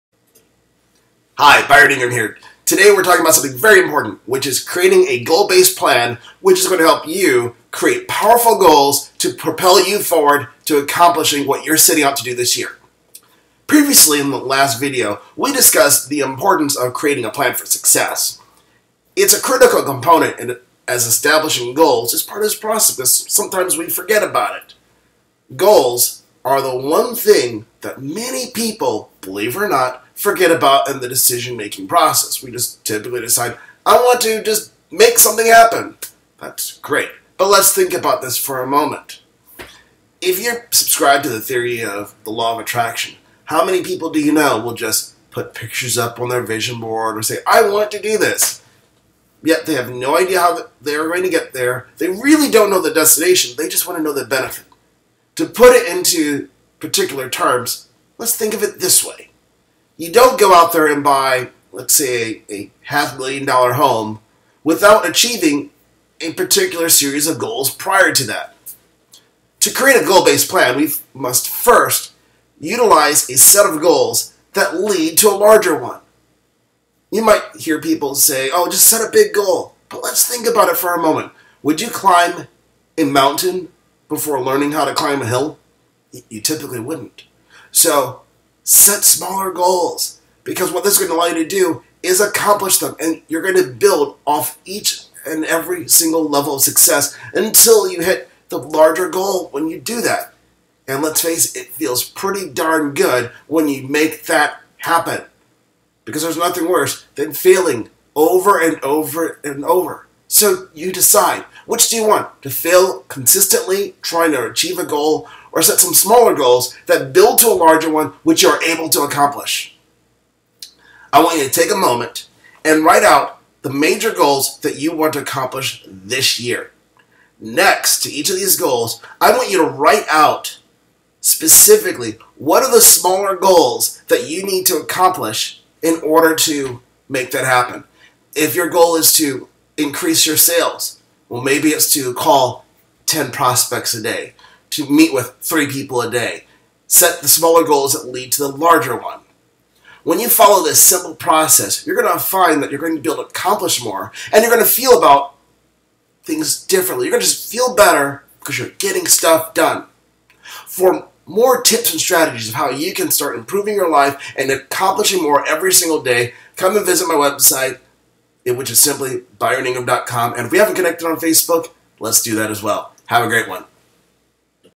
Creating A Goal Based Plan | How To Set Goals In Business & Life
Previously we discussed the importance of creating a plan for success. A critical component is establishing goals as part of the process.  Goals are one thing that many people tend to forget about in the decision making process.  Typically when people listen to things involving the law of attraction they simply write down things they want in life.  The things we want are a by product of achieving certain things.  You don't go out and purchase a $300,000 home without achieving particular goals along the way.

To create a goal based plan, we must utilize a series of goals that lead to a larger goal.  The logic for this is very simple, each smaller goal is a component of the larger one.  Because the smaller goals are quicker to achieve, it provides us with a sense of accomplishment.  Let's face it, when we accomplish something it feels pretty darn good.  But when we fail at doing something, it typically sucks!  So when you set only one goal without any incremental goals along the way, you can potentially be setting yourself up for very large setbacks.

Take a moment and write out the major goals that you want to accomplish this year.  Next on each of these goals, write out a list of sub goals that you must accomplish in order for the larger one to happen.  When you follow this simple process you will find that you are accomplishing more, and are feeling better about what you are making happen.